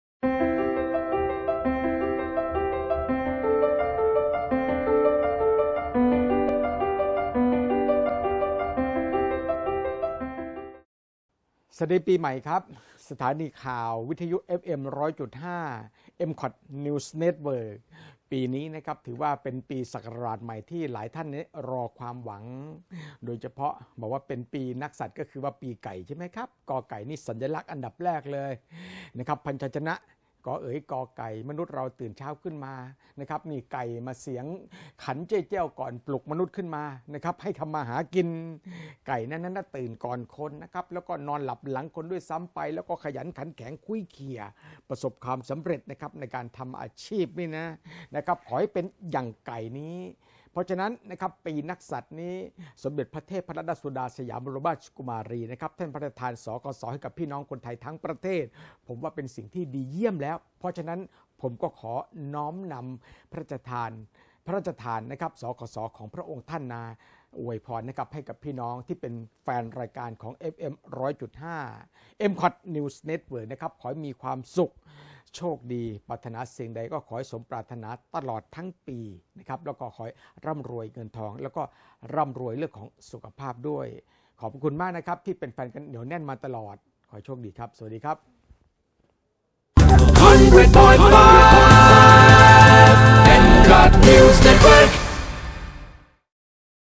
01 สวัสดีปีไก่ 2560 สุวิช สุทธิประภา
คลื่นข่าว FM 100.5 MCOT News Network โดย ผอ.สุวิช สุทธิประภา นำนักจัดรายการ Commentator ร่วมอวยพรผู้ฟังเนื่องในโอกาสวันขึ้นปีใหม่ 2560 ปีไก่ ขอให้ผู้ฟังมีความสุข ความเจริญ ร่ำรวย สุขภาพแข็งแรง และติดตามคลื่นข่าว FM 100.5 MCOT News Network ตลอดไป